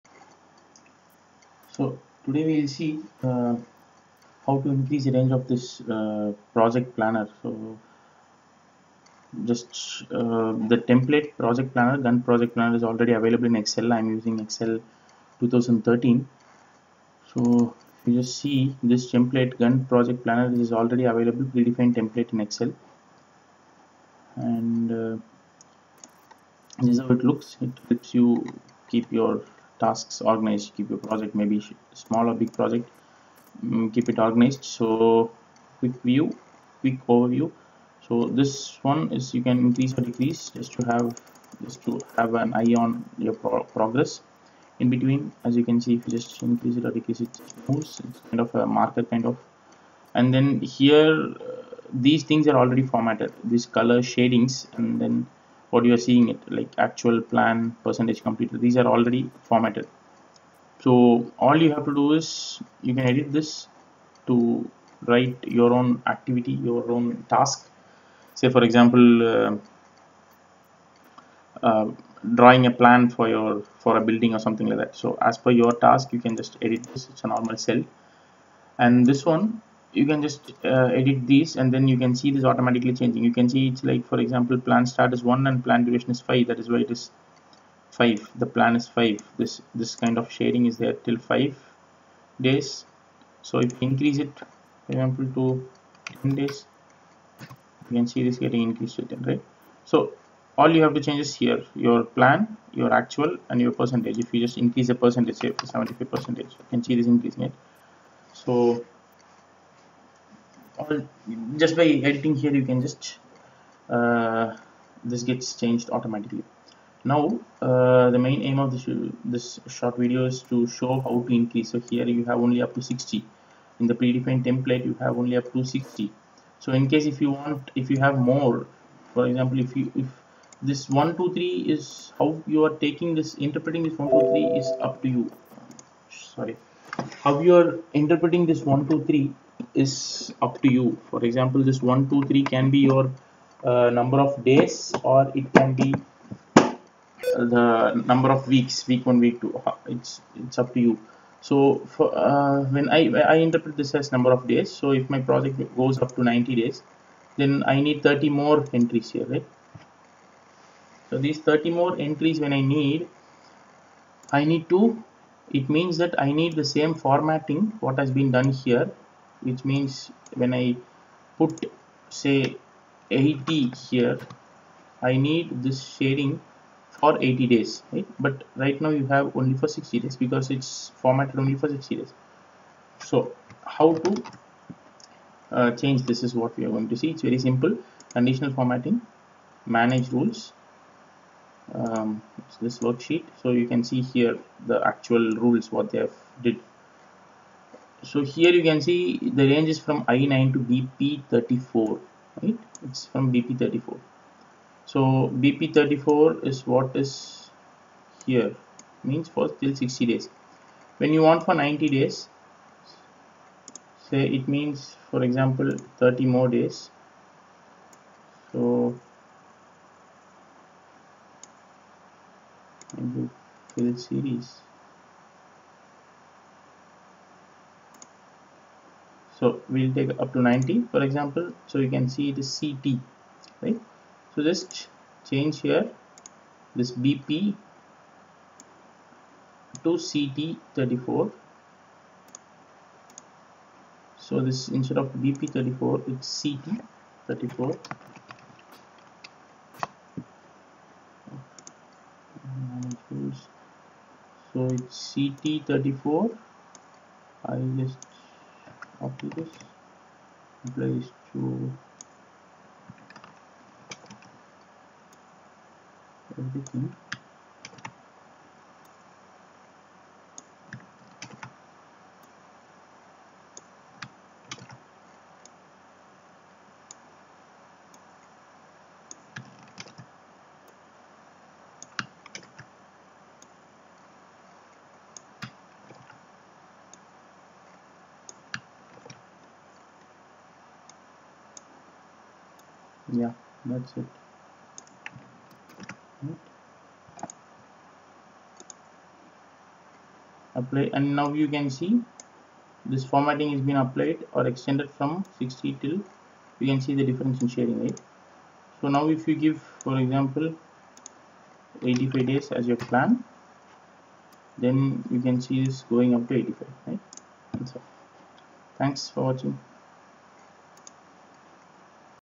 Excel 2013: Using Gantt project planner template
A short video on overview of Gantt project planner template of Excel 2013 and how to increase the range of cells